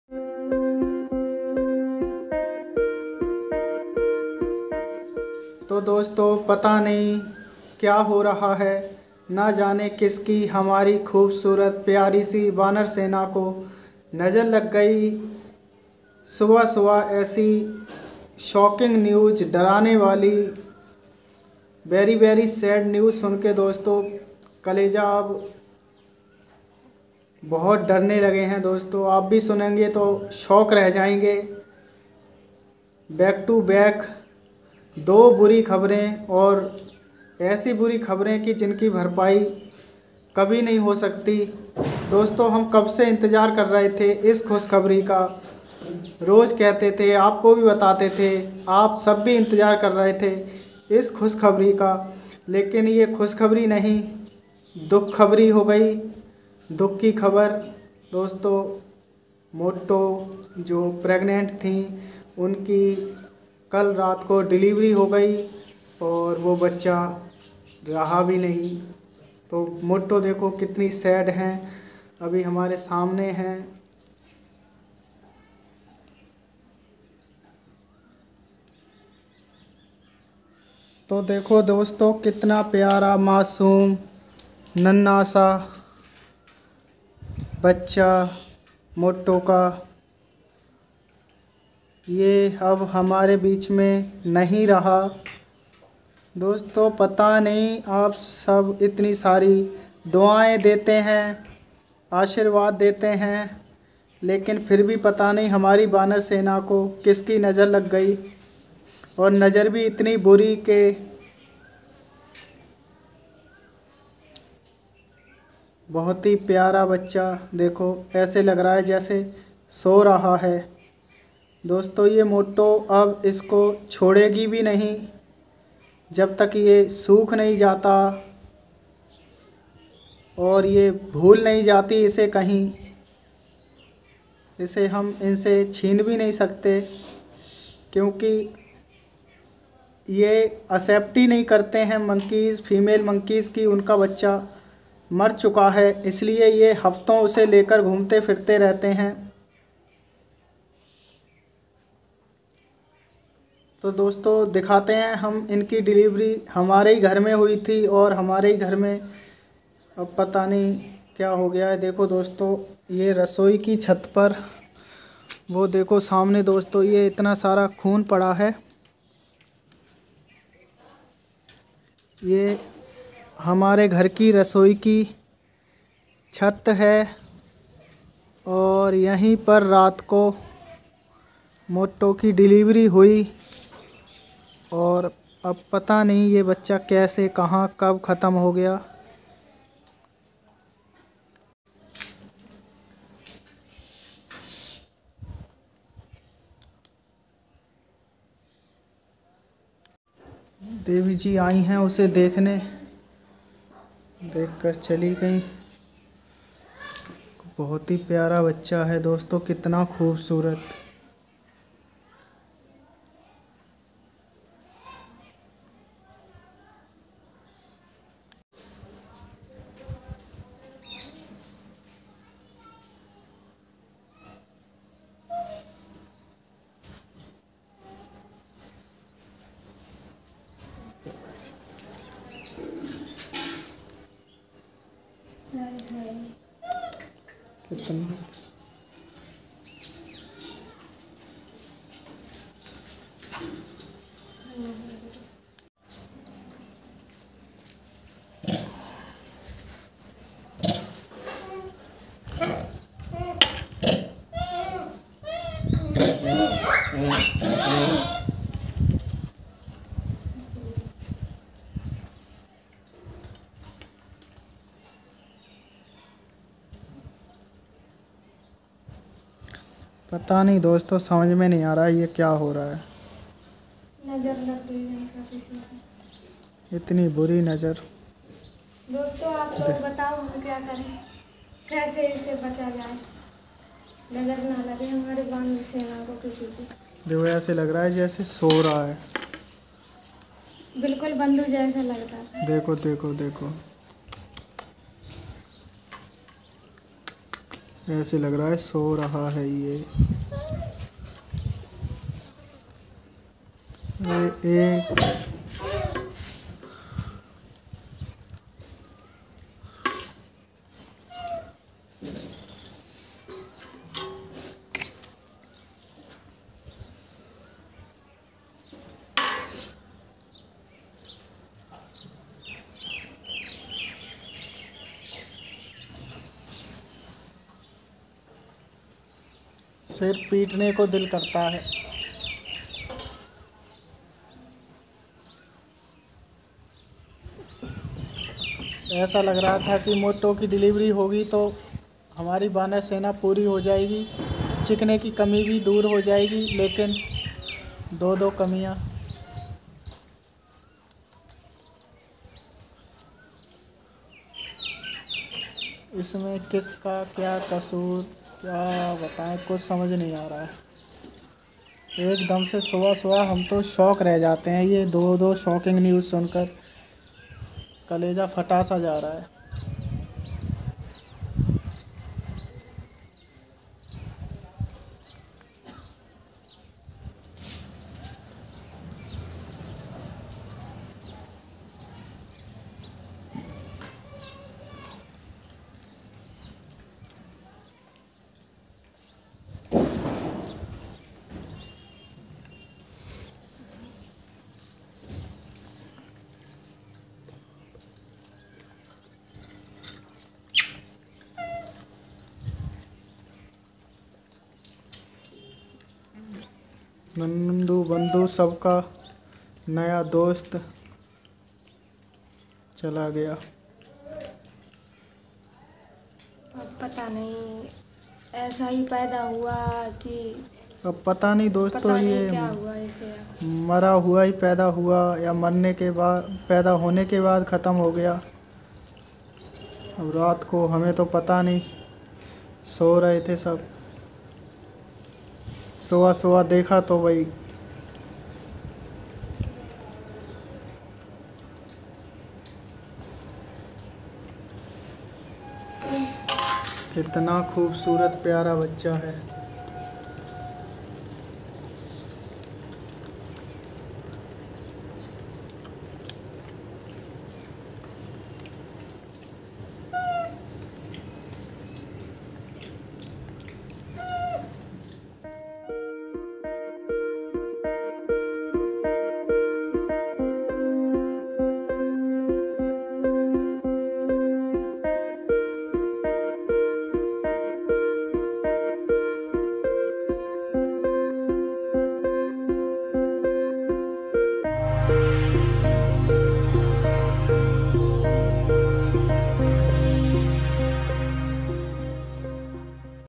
RIP To Moto's New Born Baby | Very Very Sad News | Feeding Chapati To Monkeys
Hello Guys !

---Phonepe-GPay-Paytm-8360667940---

Acc. No..34510100002034
Ifsc Code-BARB0ROPARX